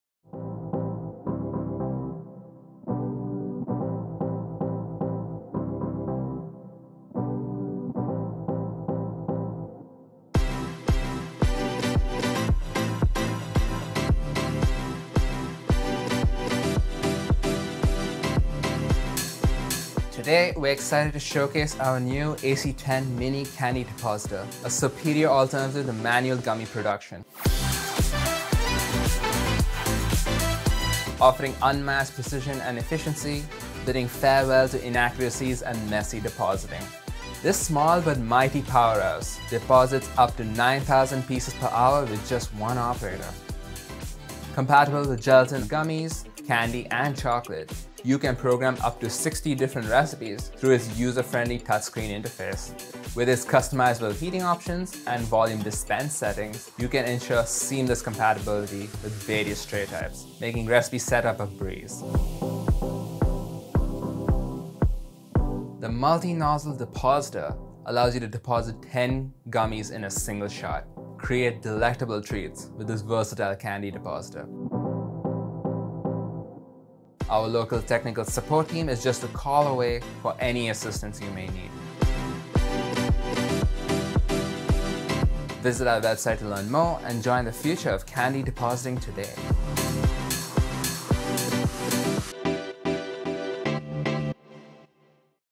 ATG Pharma: The AC10 Mini Candy Depositor
Discover unmatched precision and efficiency with the AC10 Mini Candy Depositor, bidding farewell to inaccuracies and messy depositing.

- Deposit up to 9,000 pieces per hour.
- Program up to 60 different recipes through it's user-friendly touchscreen interface.
- Only one operator necessary.
- Compatible with gelatin and chocolate.
- Customizable heating settings with two unique temperature controls.
- Volume dispense setting ensures seamless compatibility with various tray types.